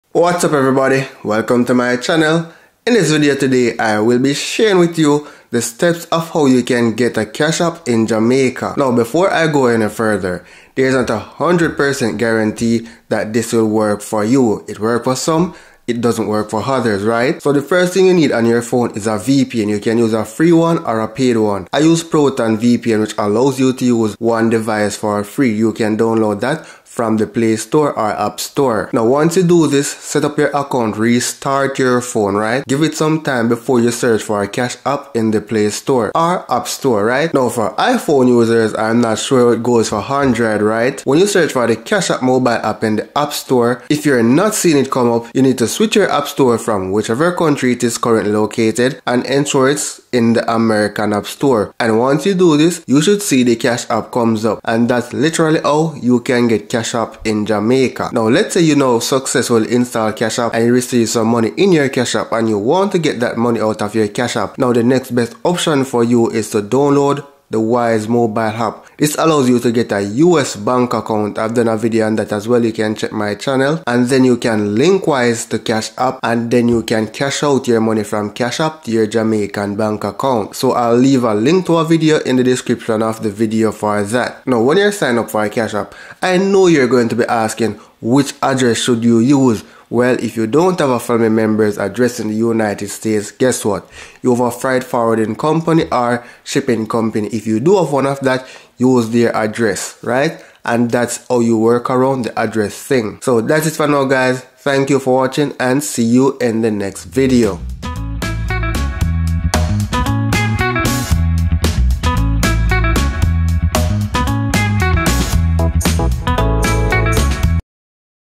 How To Get Cash App In Jamaica
This may work for some while others it doesn't but give it a try.